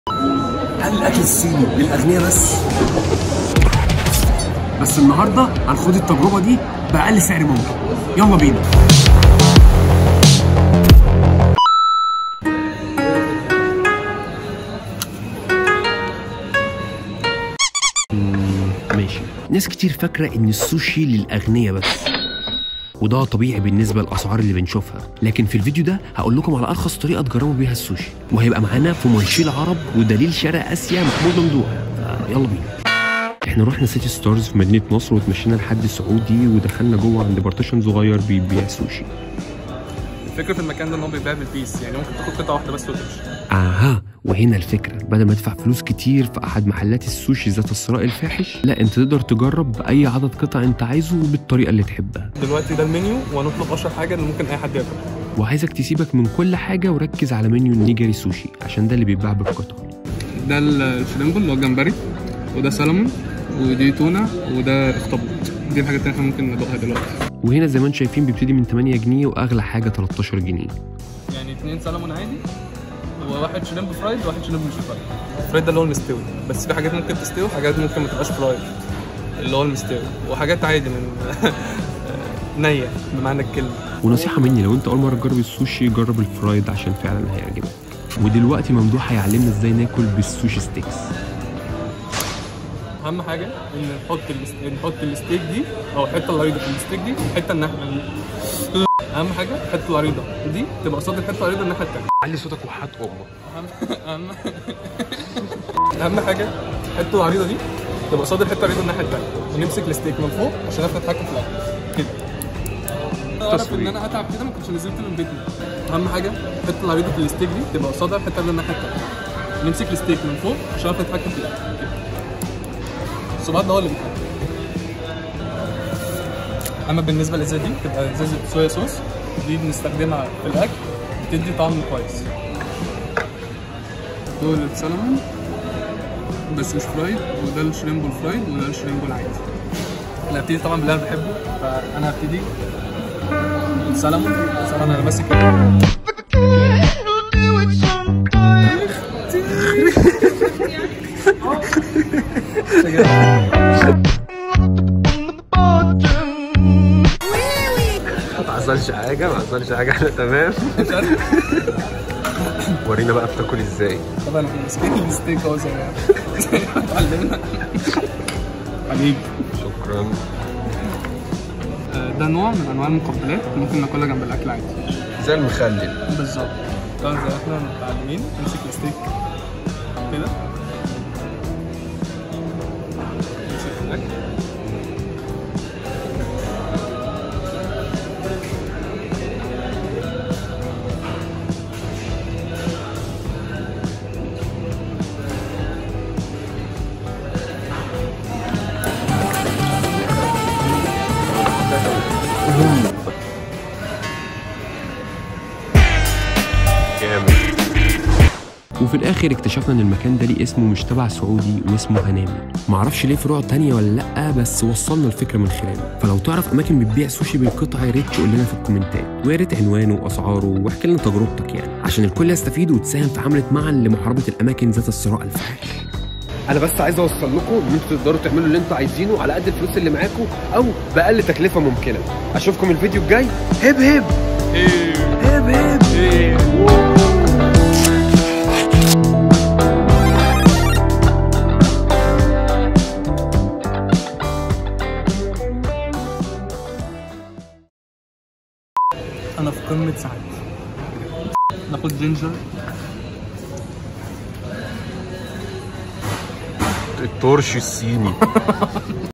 ارخص تجربة للاكل الصيني في مصر
ناس كتير فاكره ان الاكل الصيني للاغنياء بس

و دا طبيعي بسباب الاسعار اللي بنشوفها

بس في الفيديو دا هنخوض تجربة ارخص اكل صيني في مصر

الجزاء التاني من تجربة النودلز و التشكين سويت اند سور (الفراخ المسكره)